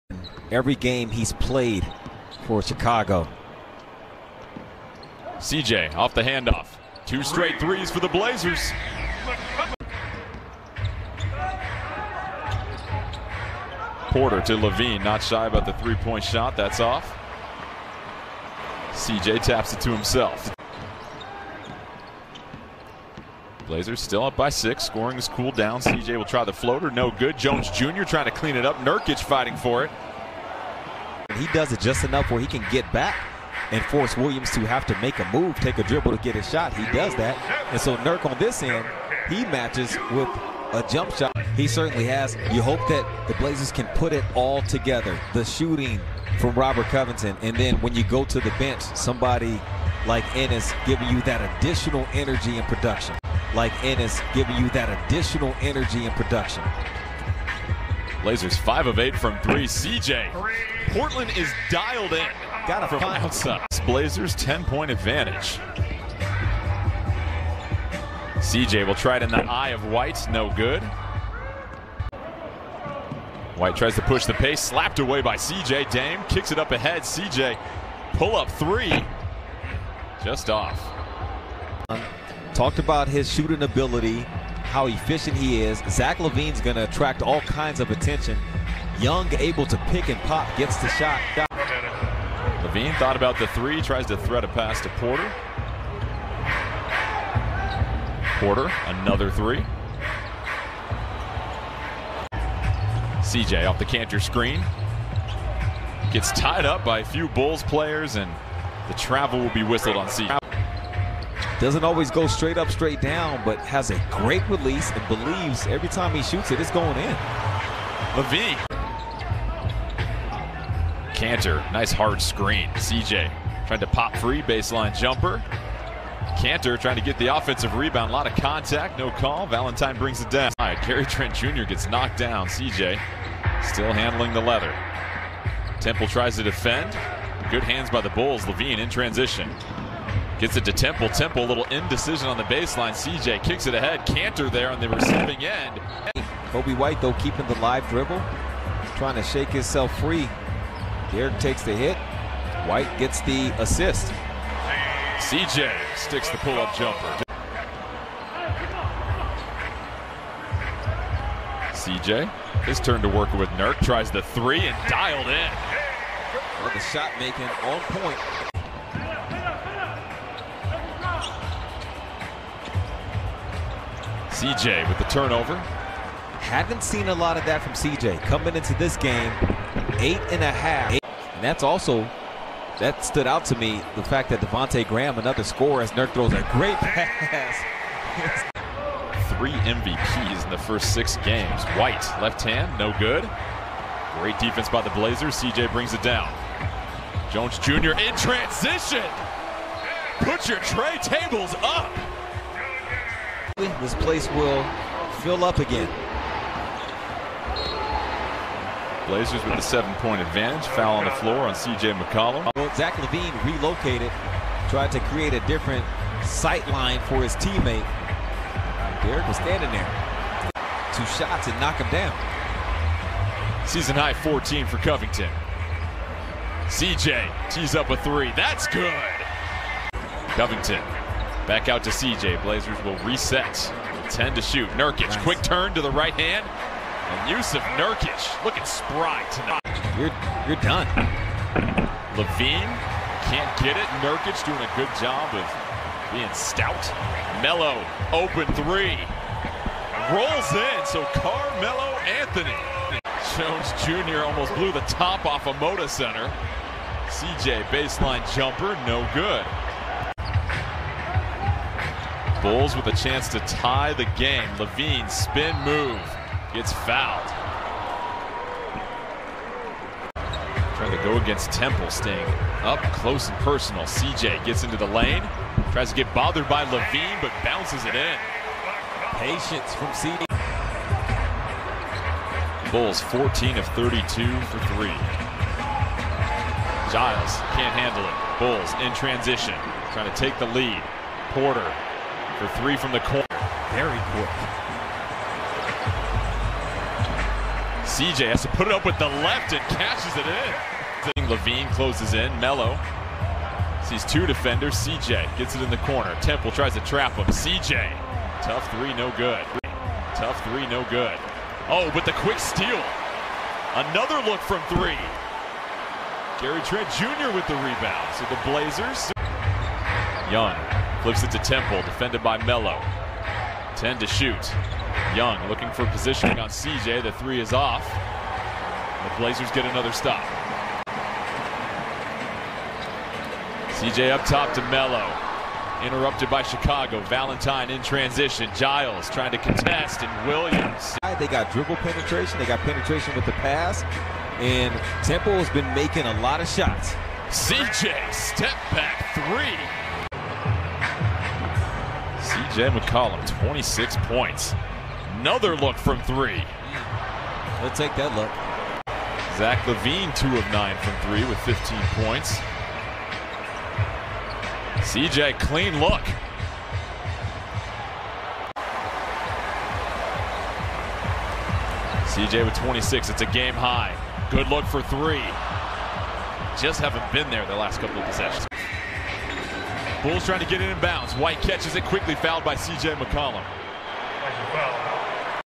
CJ McCollum 26 PTS: All Possessions (2021-01-05)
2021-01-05 Portland Trail Blazers vs Chicago Bulls: Player Highlights CJ McCollum
    26 PTS, 5 REB, 4 AST, 3 STL.
        6 of 14 from 3.
        10 of 21 FGs in 35:57 minutes.